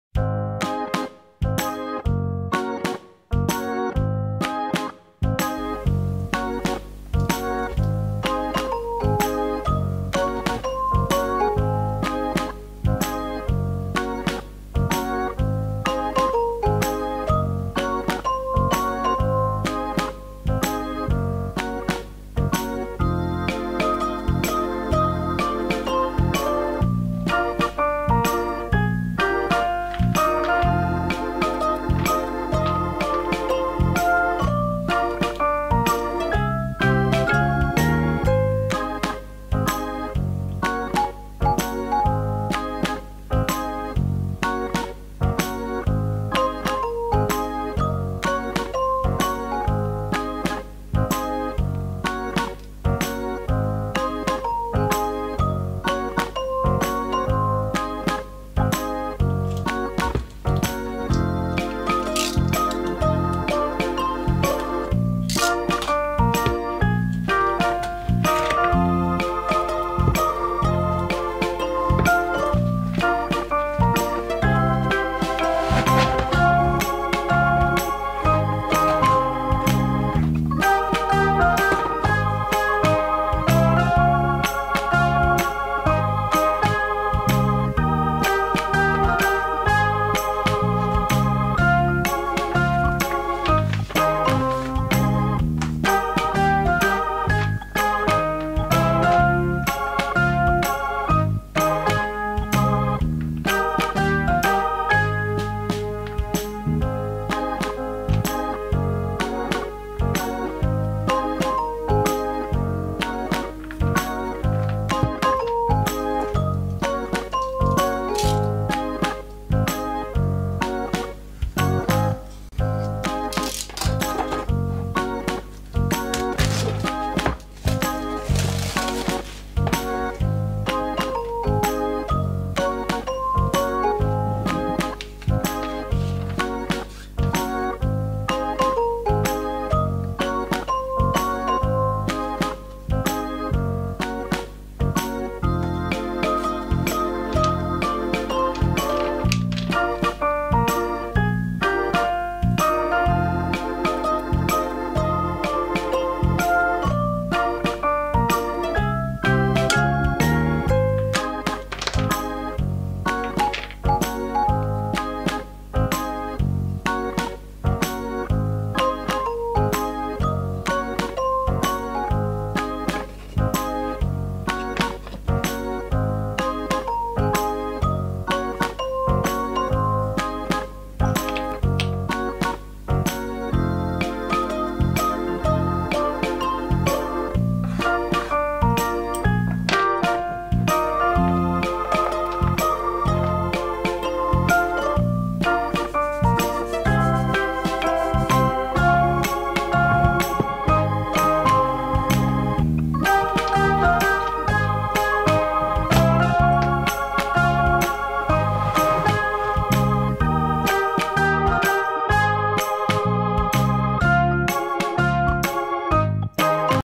PlayTive Toy Laptop Junior Wooden educational toy UNBOXING (Lidl 85 items 78 magnetic)
Delivery contents
1 laptop
78 magnetic letters, numbers and mathematical
symbols
1 wooden cell phone
2 chalks
1 sponge
2 printed sheets
1 instruction for use

IAN: 322994

Features Toy Laptop:
through play supports the first learning to count and write
78 magnetic letters, numbers and mathematical symbols
with telephone and writing board
a sponge and 2 chalks included
a review of pictures and numbers encourages fun
made of natural wood
recommended age of the child: from 3 years old
85 items